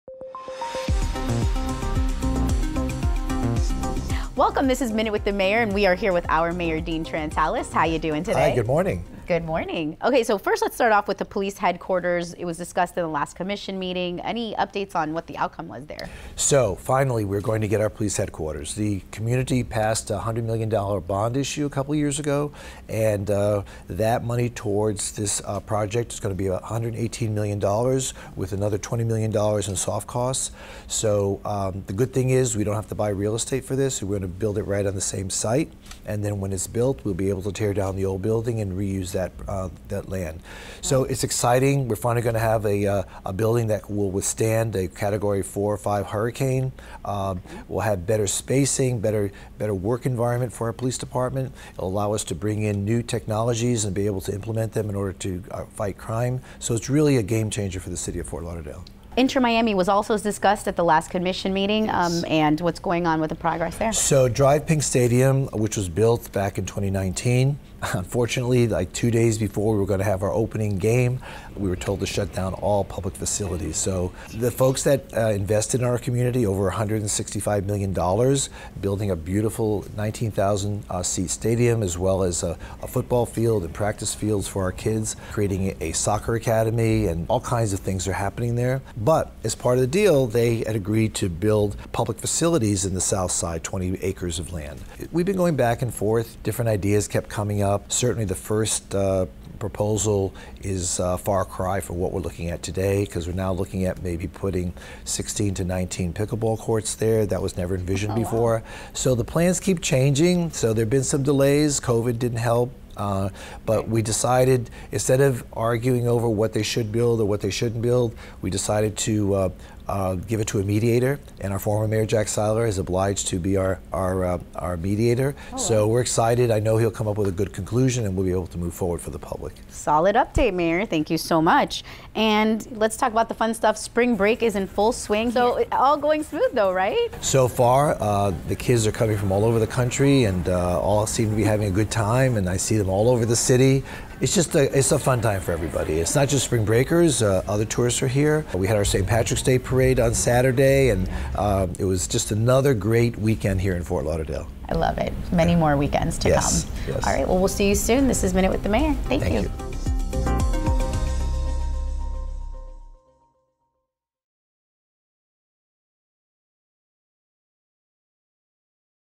Minute with the Mayor- 3/15/2023
Want to hear a rundown of what’s happening in the City of Fort Lauderdale in just a few mins? This week's Minute with the Mayor ⏱️ shares:
🏛️ HQ Progress
🏛️ Progress on the park at Drive Pink Stadium,
🏛️ Spring Break